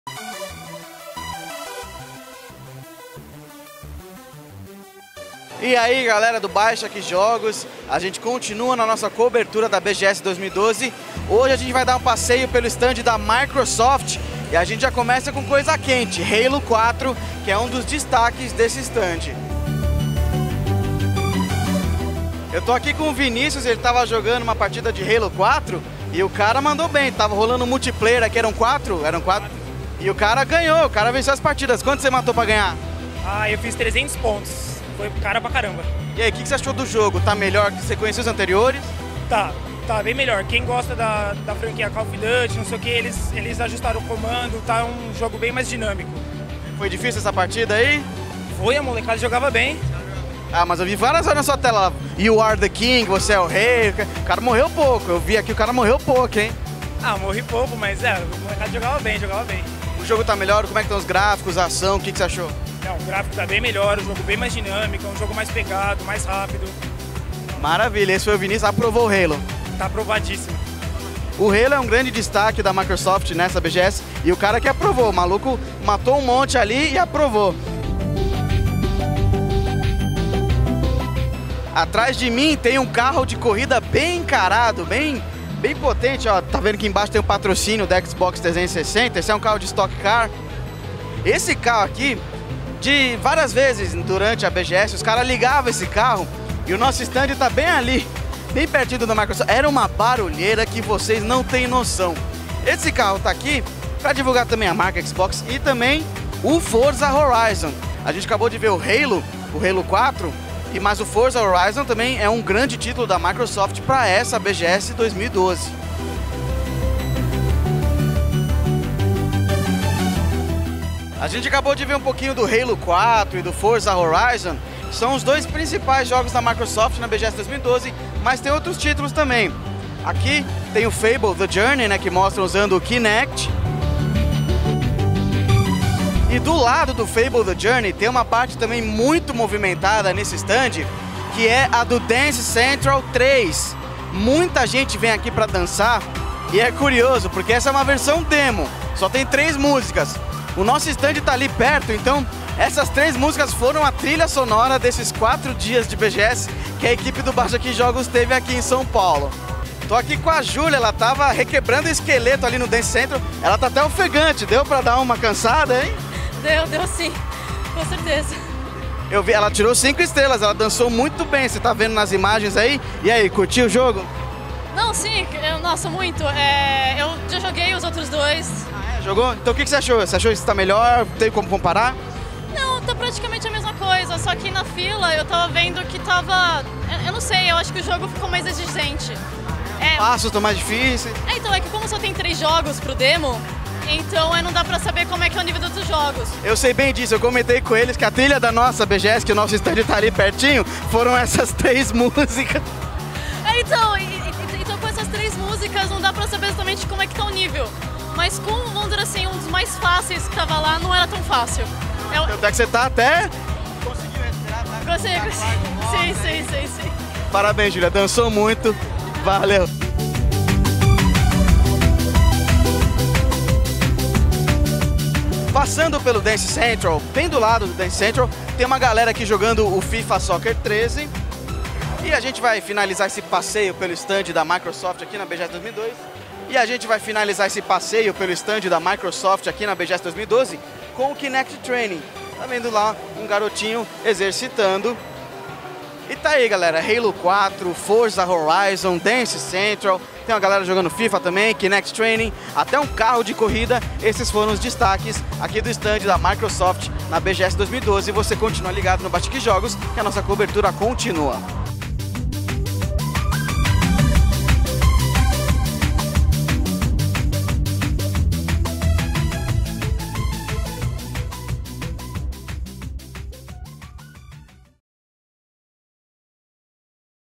Microsoft: destaque para Halo 4 e muito mais [BGS 2012] - Baixaki Jogos
Chegou a vez de passarmos pelo estande da Microsoft. Demos uma boa olhada em Halo 4, Forza Horizon e até no novo Dance Central 3. E de quebra, conversamos com algumas pessoas para saber o que acharam das novidades.